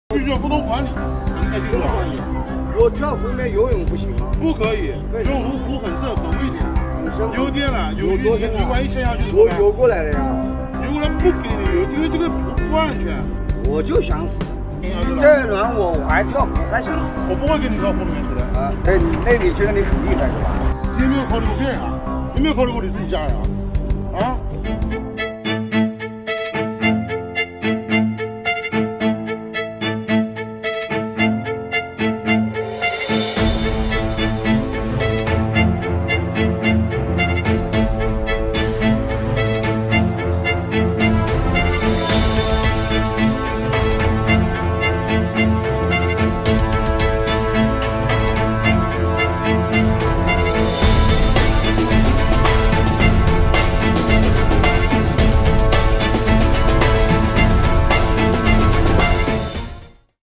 南京一大爷湖中游泳不听劝阻，不配合且振振有词：我就想死！
江苏大爷岂能甘居人后！！！！